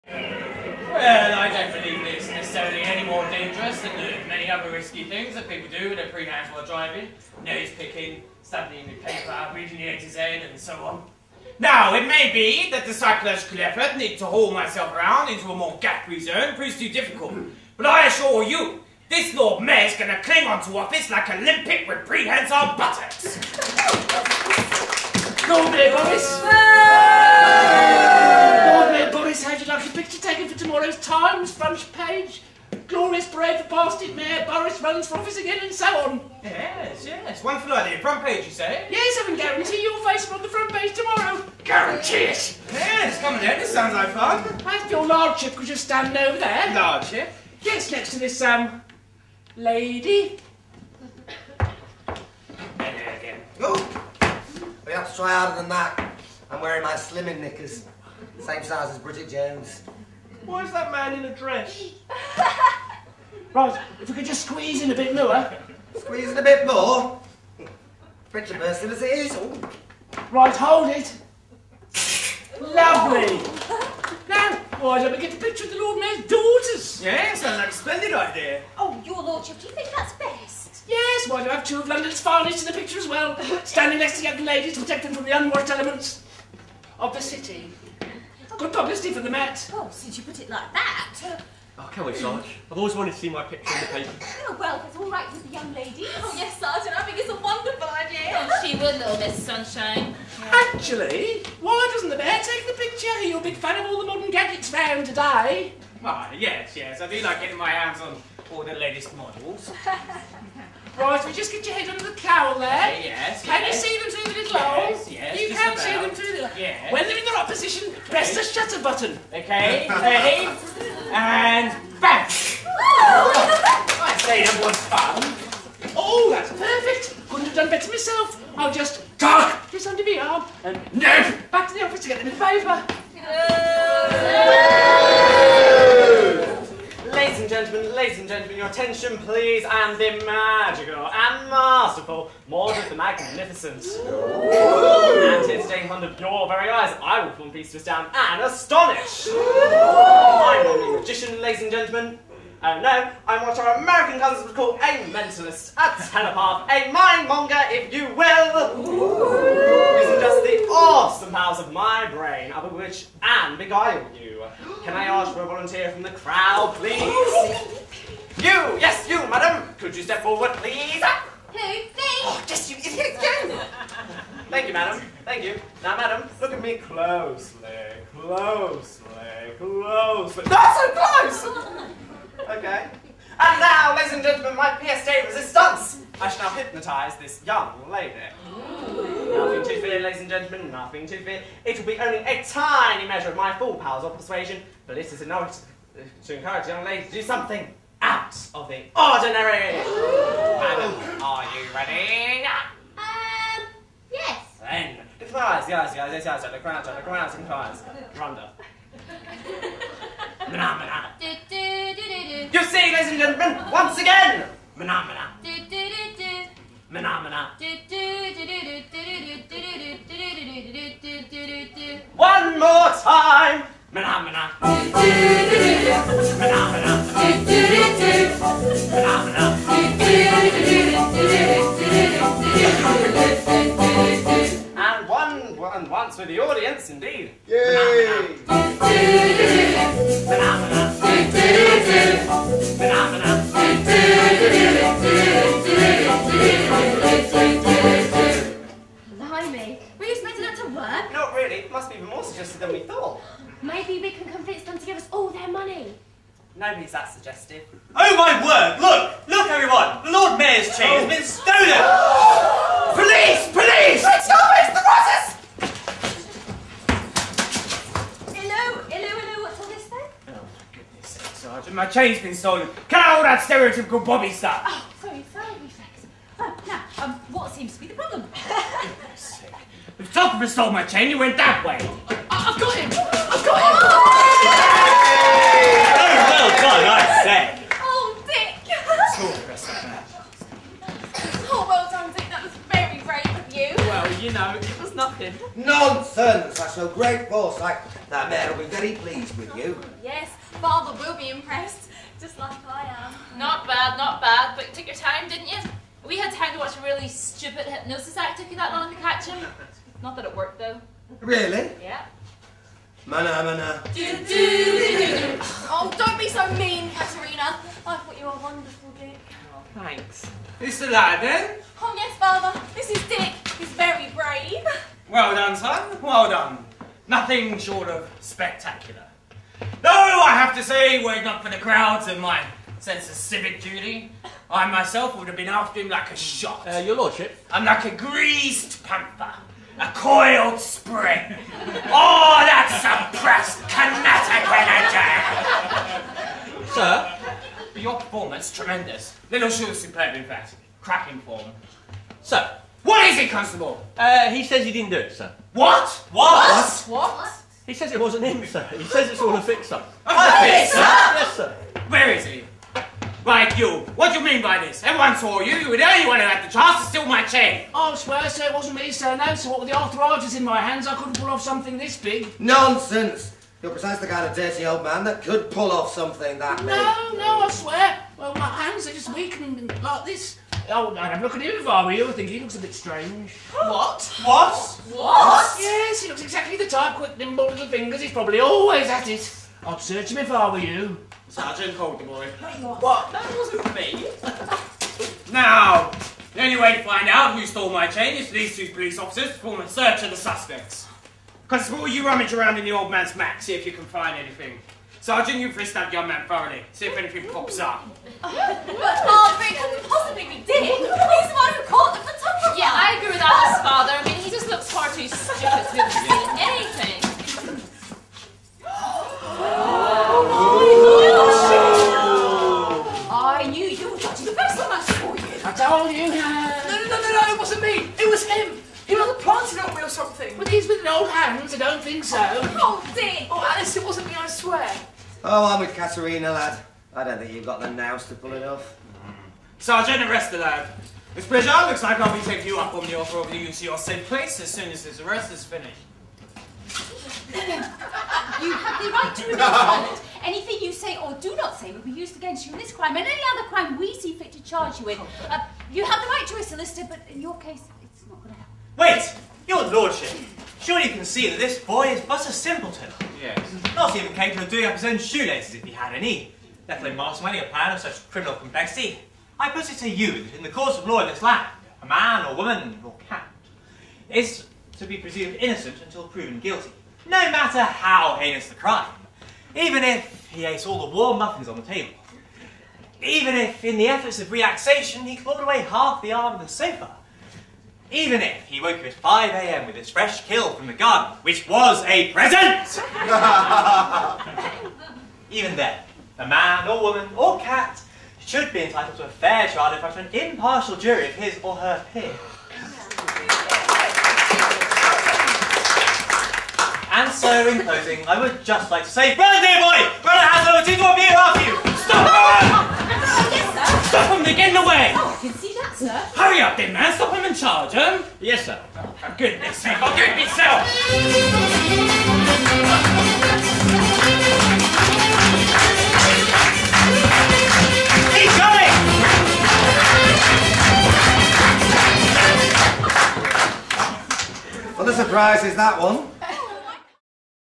parade.mov
The Lord Mayor's Parade scene from South London Theatre's 2009 Panto 'Dick Whittington', written and directed by Charles Doyle